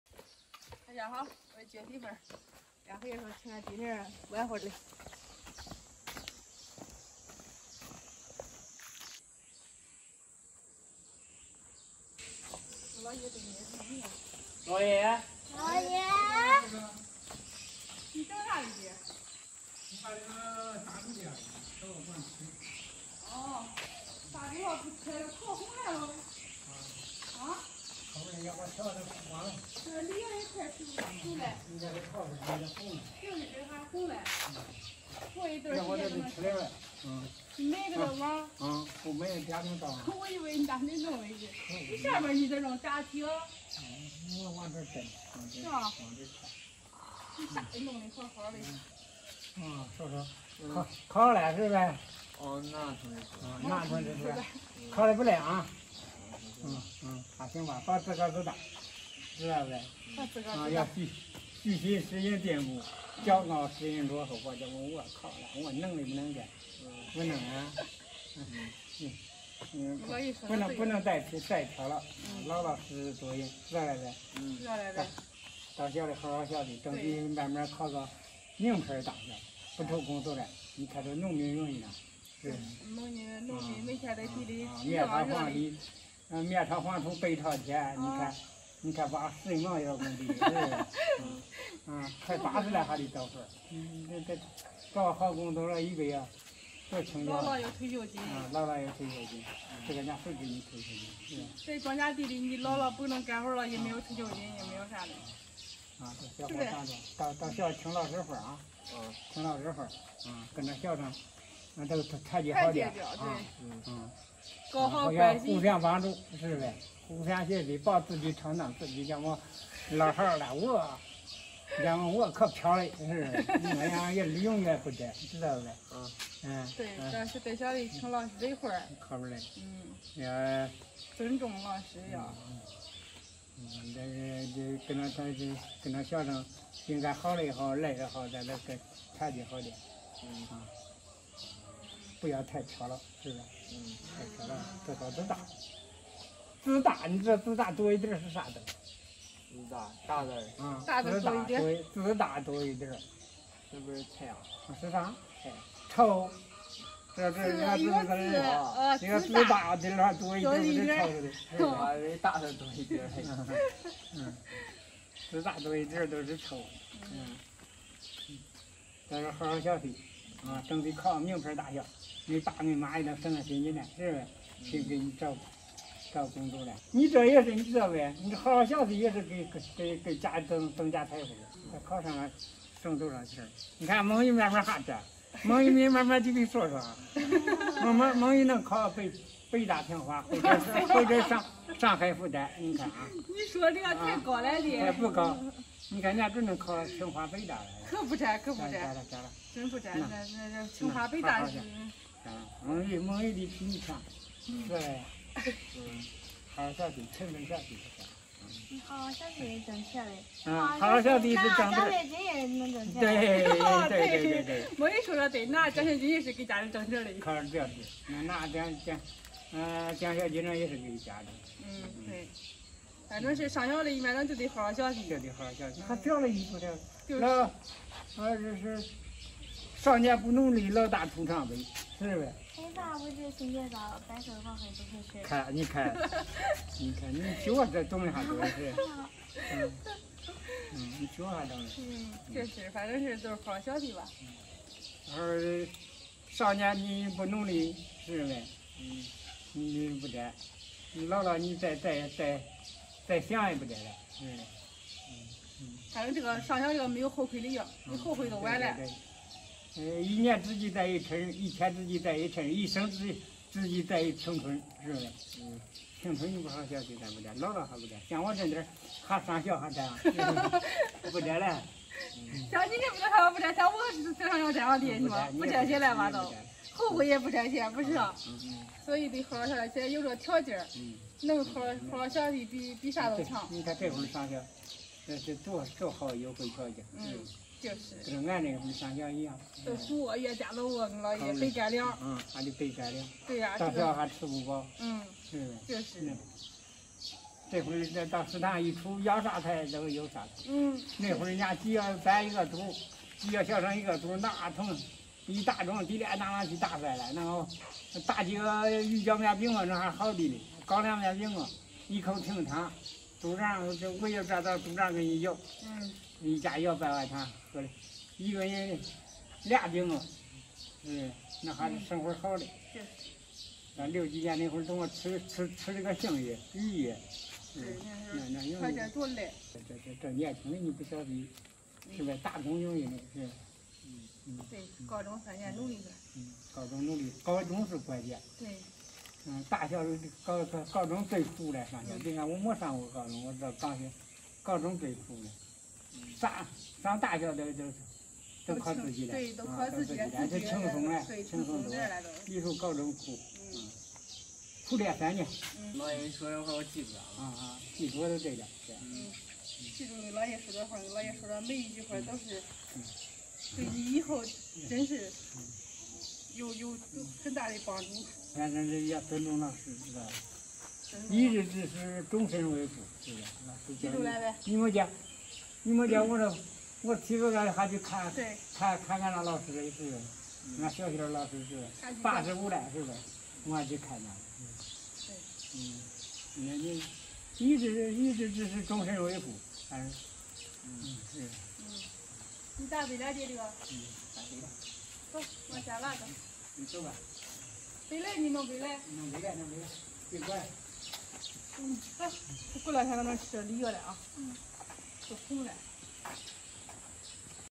硕硕报完名回家，老爸言传身教讲道理，告诉硕硕做人要充满正能量【倔媳妇】
硕硕最近走到哪里都是焦点，这不是在姥爷面前，乖乖听道理~~